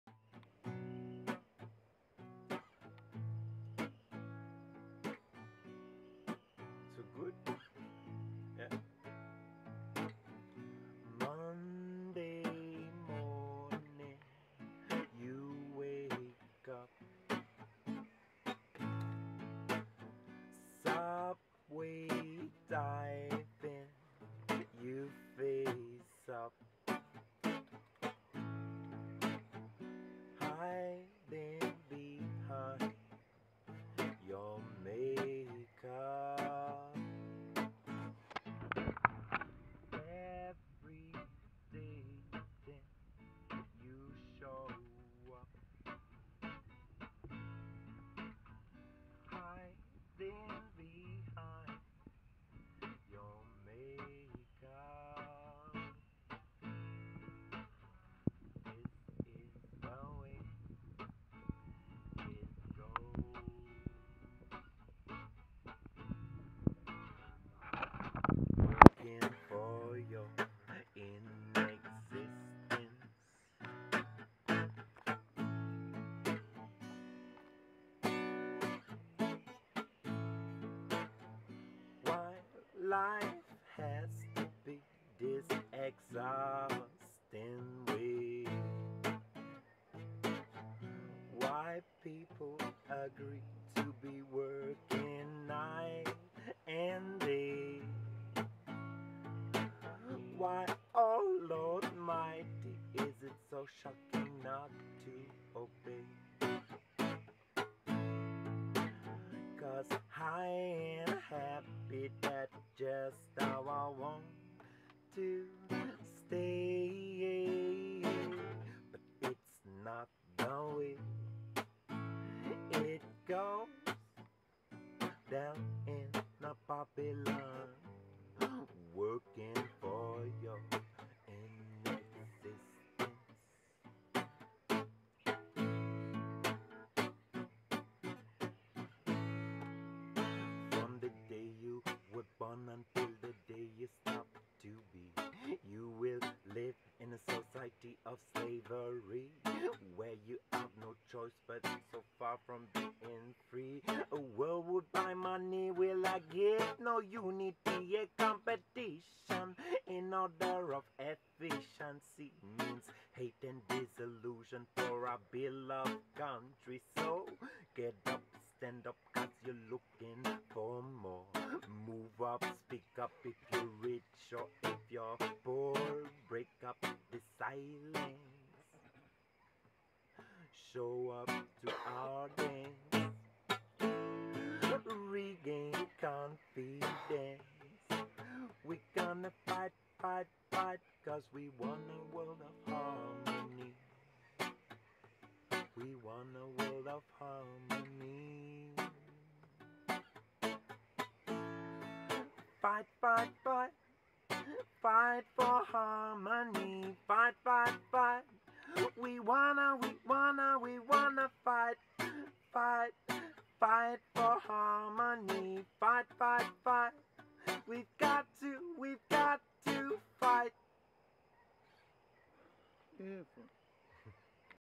Steve Riols; reggae man from paris in the swamps of Byron; candelight reggae... low light; new camera lessons with some tough audio learns, apologies, thanks, photage shot nikon D5100 at ISO 3200.